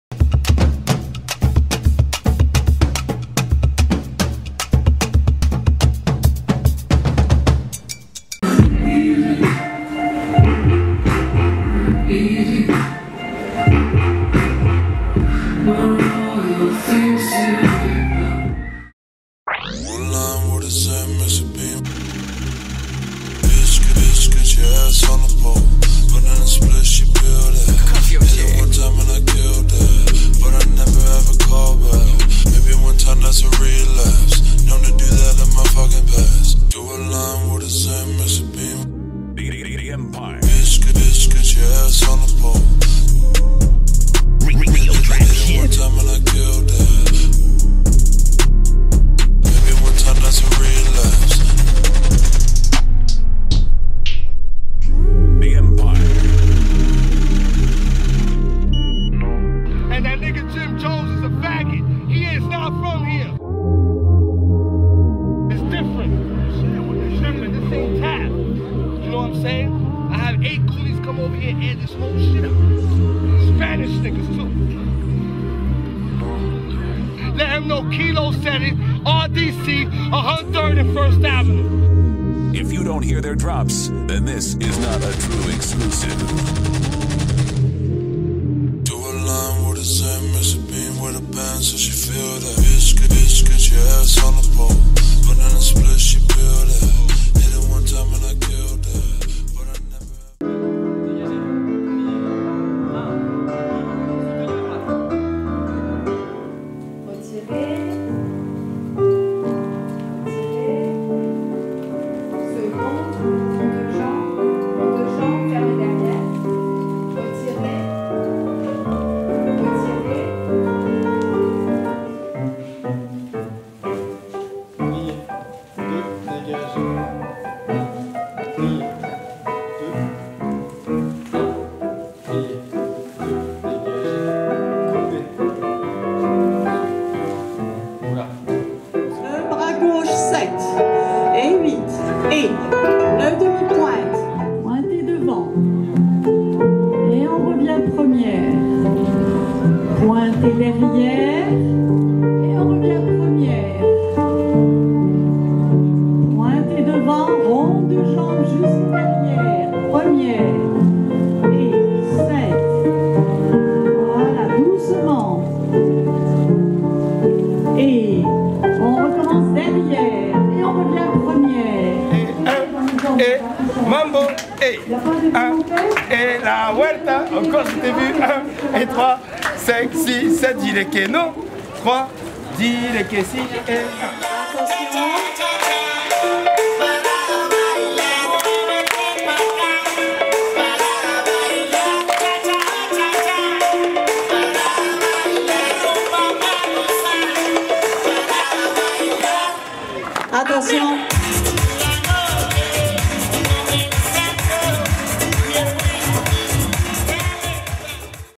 2éme biennale de danse 28 et 29 janvier 2017
Gros succès populaire pour cette 2éme Biennale de danse qui a eu lieu à Saint Pierre les 28 et 29 janvier 2017.